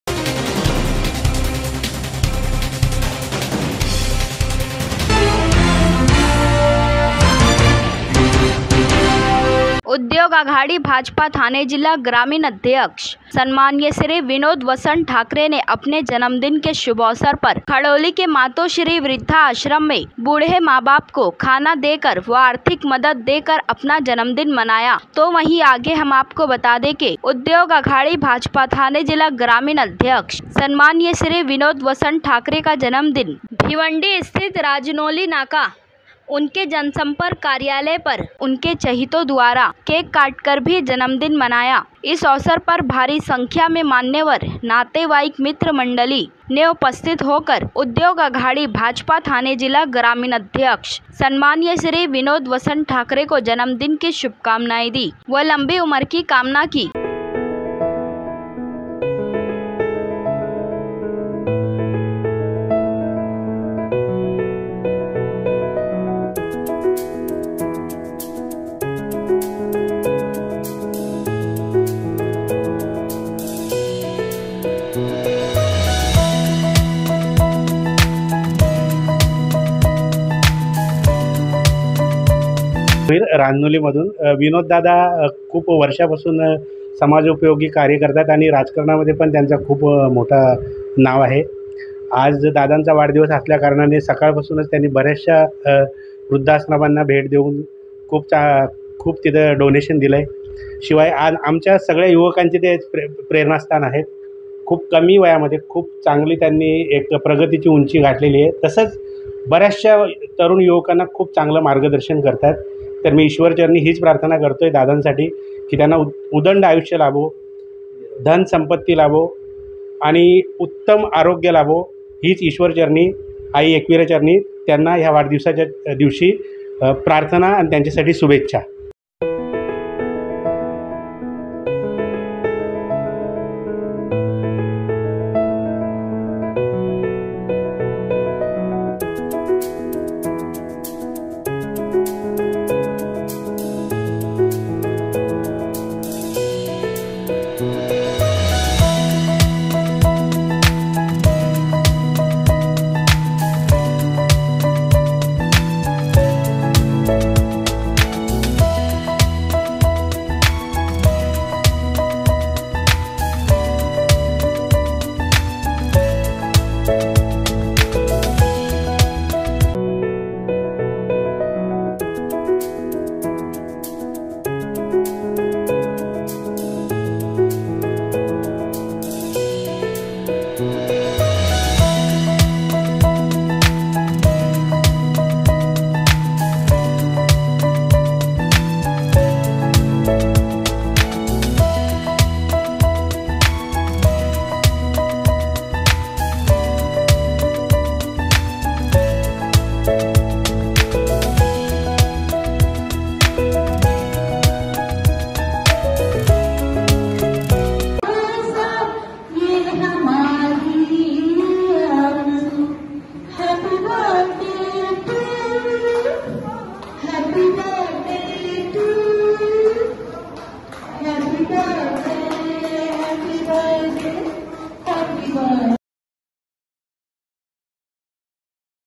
उद्योग आघाडी भाजपा ठाणे जिल्हा अध्यक्ष विनोद ठाकरे का जन्मदिन उनके चहीतो द्वारा केक काटकर मनाया
उद्योग आघाडी भाजपा ठाणे जिल्हा ग्रामीण अध्यक्ष विनोद वसंत ठाकरे का जन्मदिन उनके चहीतो द्वारा केक काटकर मनाया